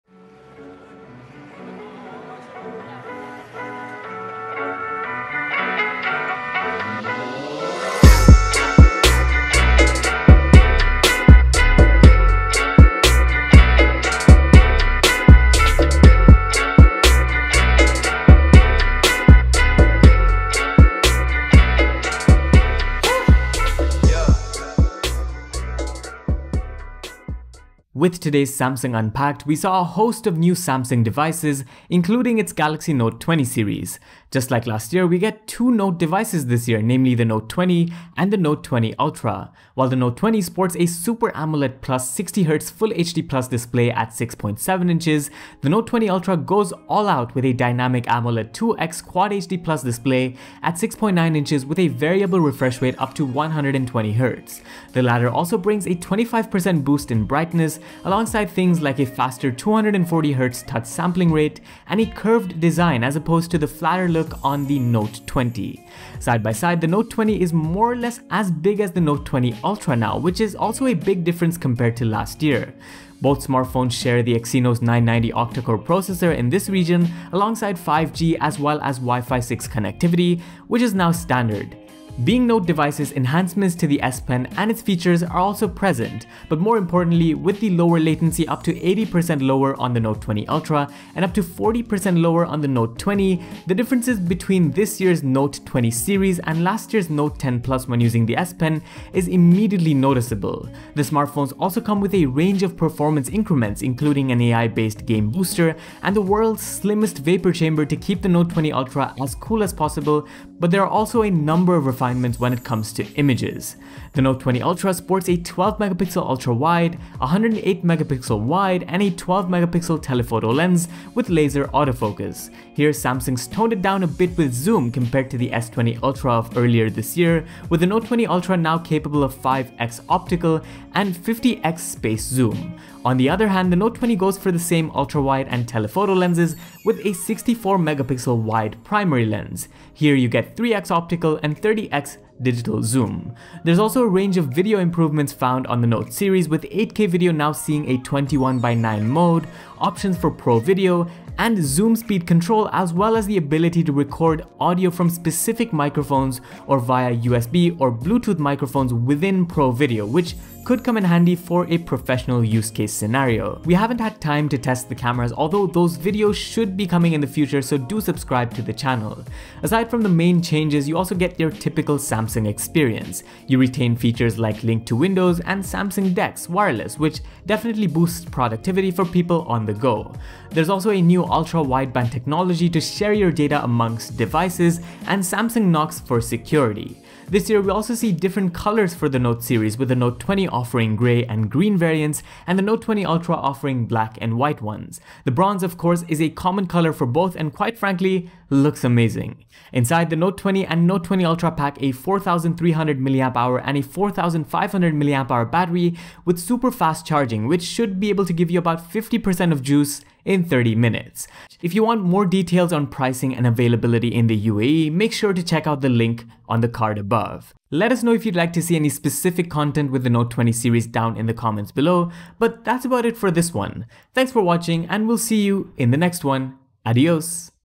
Samsung Note 20 Series - First Look!
This is the first look at the Samsung Note 20 and Note 20 Ultra from today’s Unpacked virtual event

Video and Edit: Daanesh Kalyaniwalla

Voice-over: Vaibhav Pradip